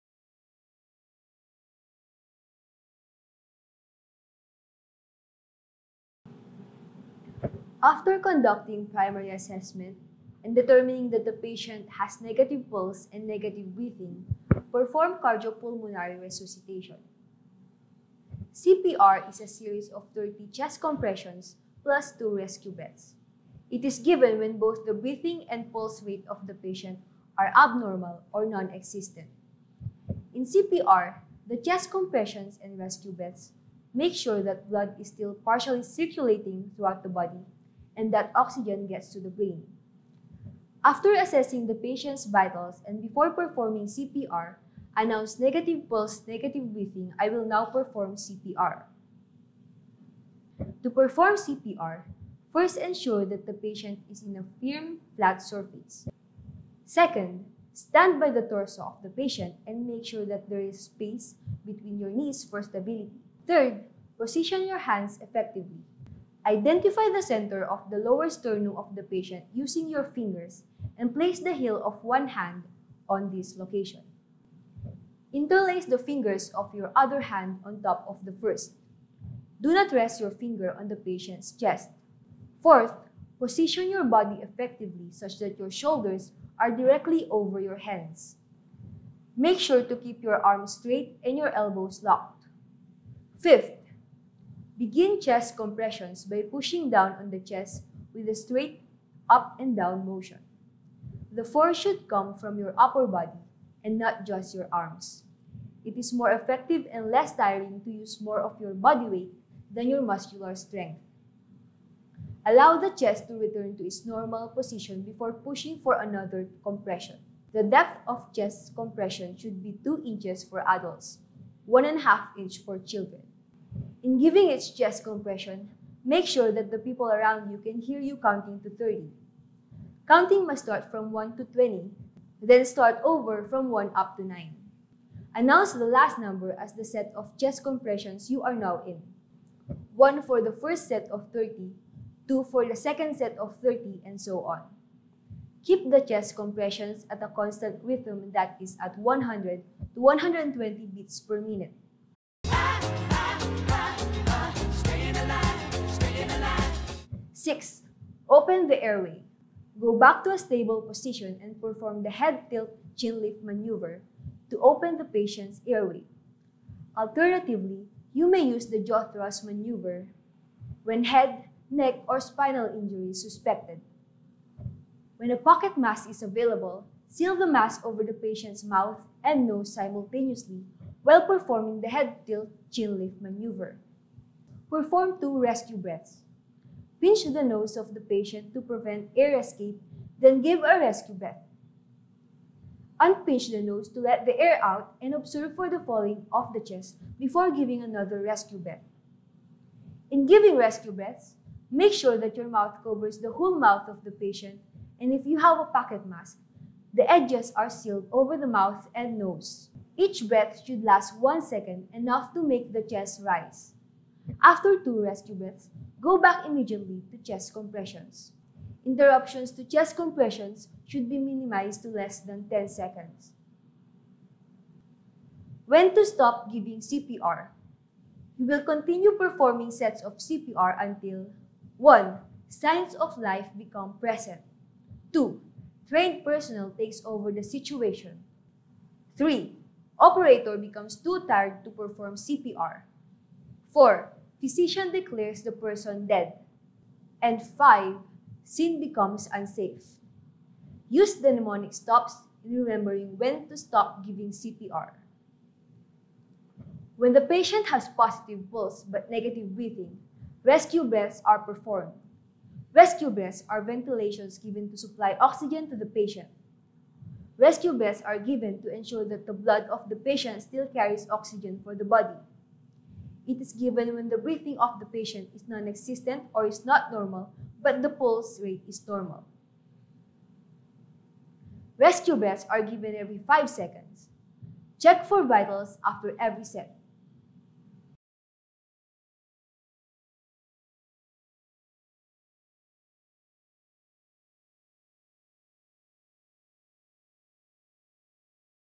UP ROTC - Cardiopulmonary Resuscitation | First Aid
This video focuses on demonstrations of cardiopulmonary resuscitation or CPR.